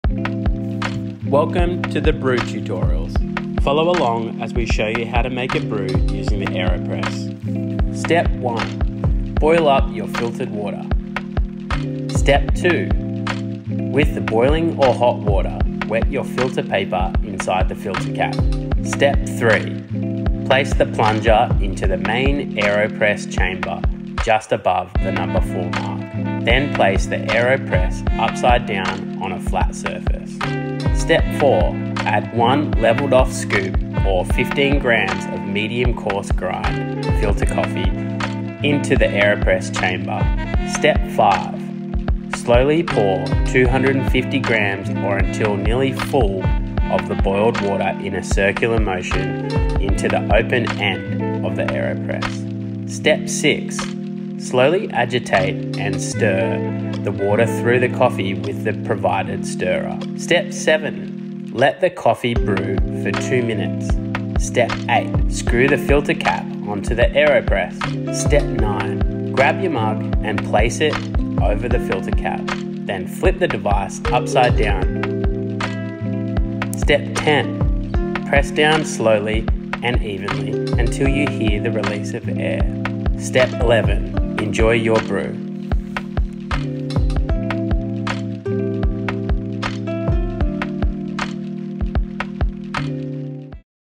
AeroPress Brew Tutorial | Blackboard Coffee Roasters
Welcome to the Blackboard Coffee Roasters Brew Tutorials. Follow along as we show you how to brew with an AeroPress!

HOW TO MAKE AN ‘AEROPRESS’ COFFEE
1. Boil filtered water
2. With the boiling, wet your filter paper inside the filter cap
3. Place the plunger into the main AeroPress Chamber just above the #4 mark, then place the AeroPress upside down on a flat surface.
4. Add one levelled off scoop, which is provided with your AeroPress, or 15g of medium course grind Filter Coffee into the AeroPress chamber.
5. Slowly pour 250g or until nearly full of the boiled water in a circular motion into the open end of the AeroPress
6. Slowly agitate & stir the water through the coffee with the provided stirrer
7. Let the Coffee brew for 2 mins
8. Screw the filter cap onto the AeroPress
9. Grab your mug & place it over the filter cap, then flip the device upside down so the mug is now on a flat surface
10. Press down slowly & evenly until you hear the release of air
11. Enjoy your brew!

BREW GEAR LIST:
- AeroPress Coffee Maker
- AeroPress Filter Papers
- AeroPress Stirrer, or teaspoon
- Kettle, or boiling water
- Your Choice of Coffee - We recommend Roaster's Pick Filter with a Medium Course Grind
- Your fav Mug, or Carafe
- Drip Scales